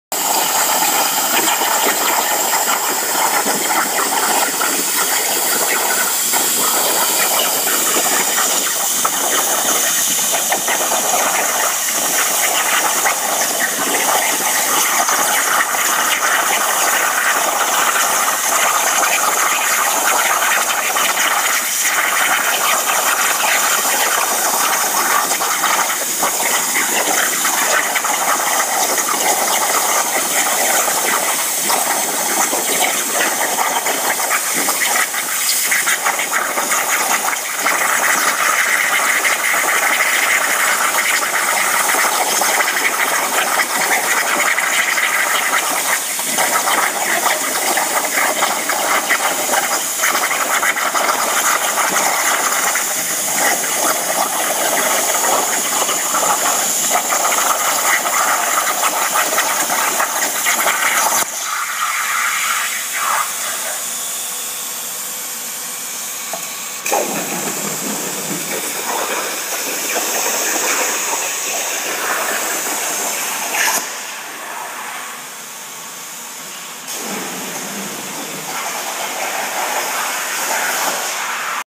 Water table test
After dealing with smoke, metal dust and warpage for the past year, I built a water table. Works great!